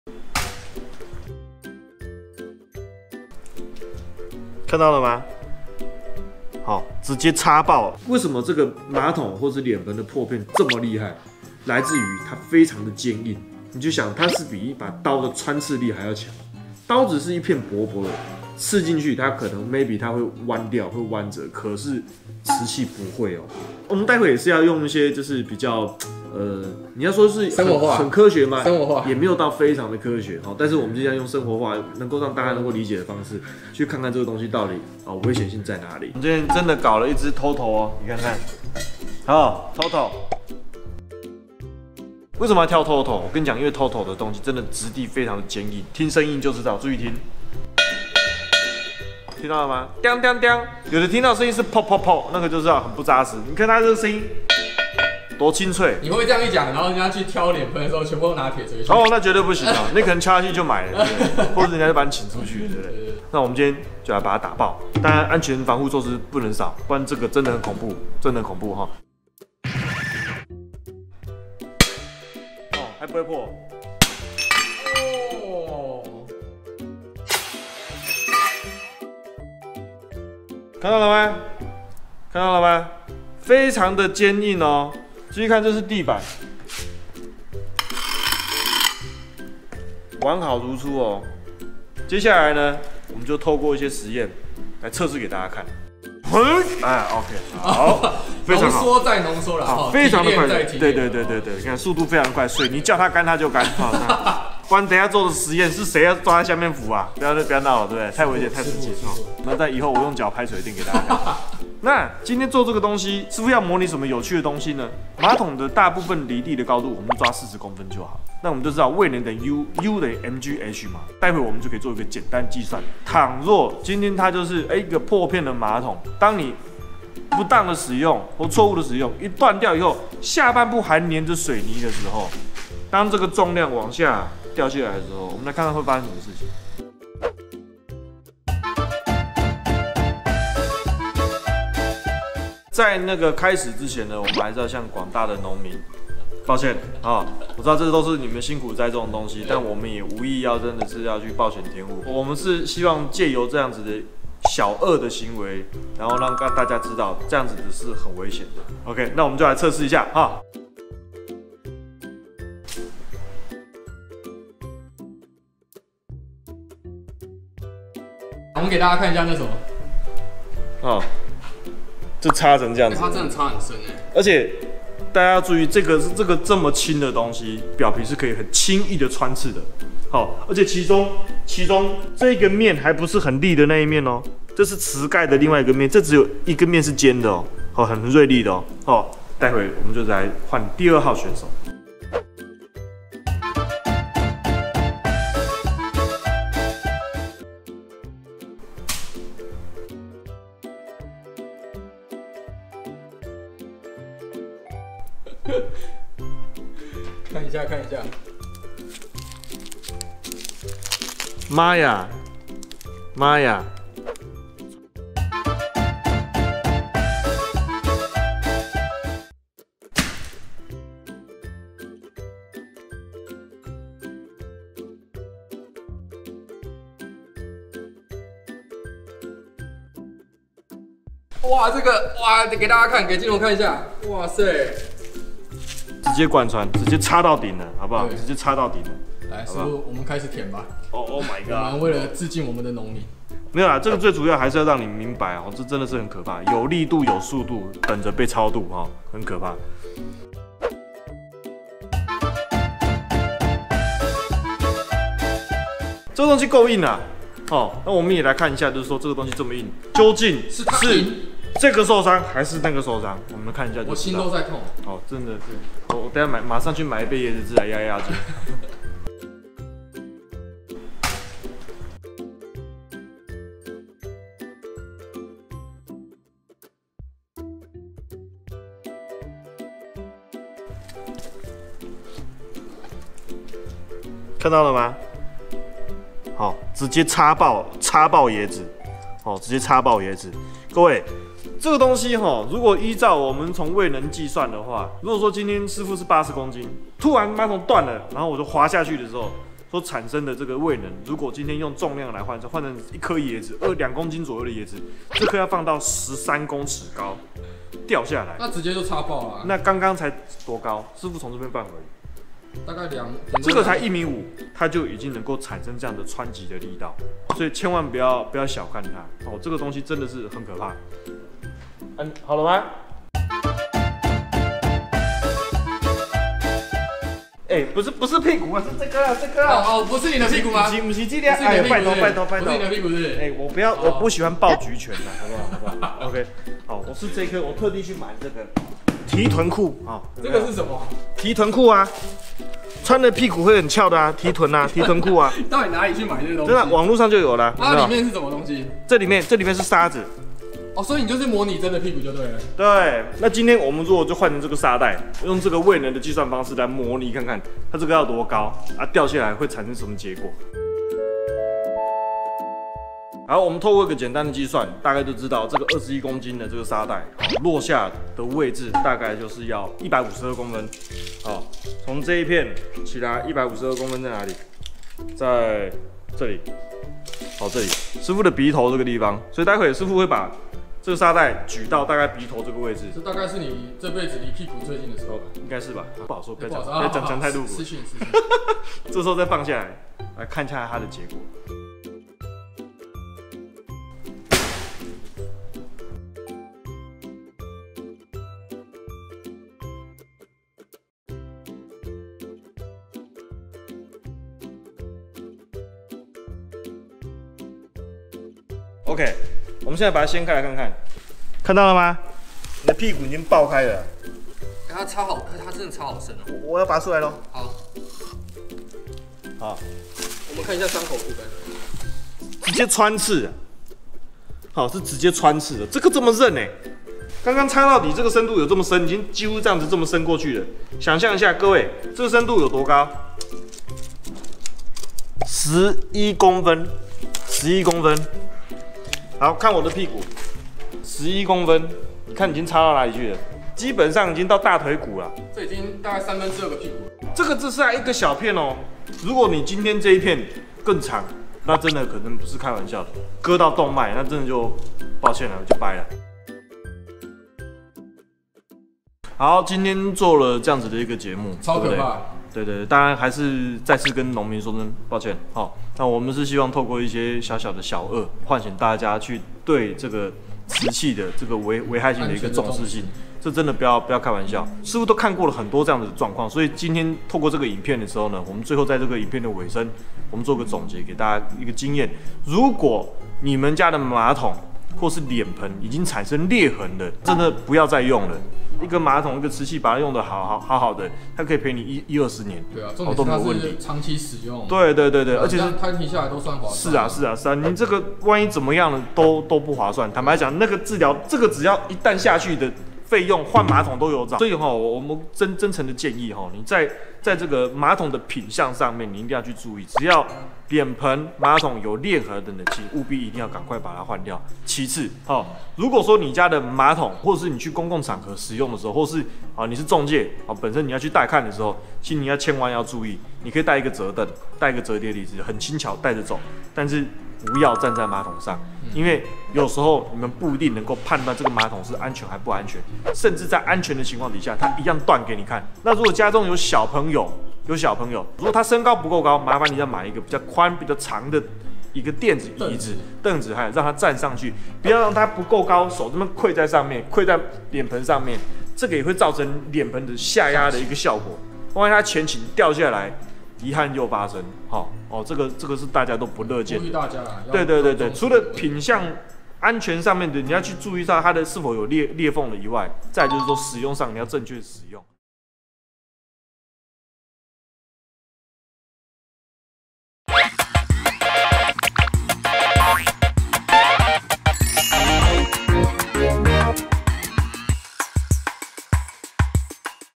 馬桶、洗手台碎片比刀還利！水電師傅實測椰子秒被穿破爆汁｜浴室｜陶瓷
☑影片授權：@NHRepair